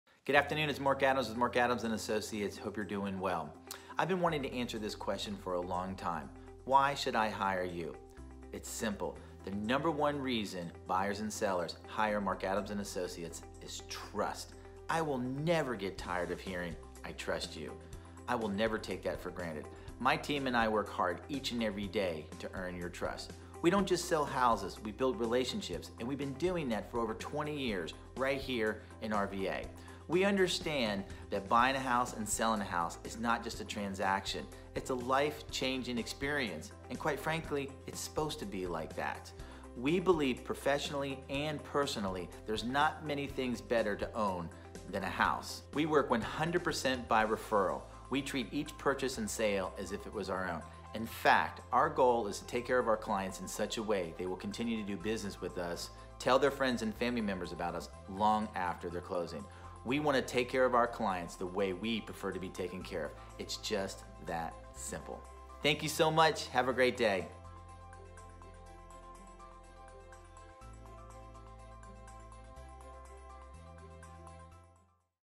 Why Should You Hire Me?
Mark Adams explains the top reasons for working with Mark Adams & Associates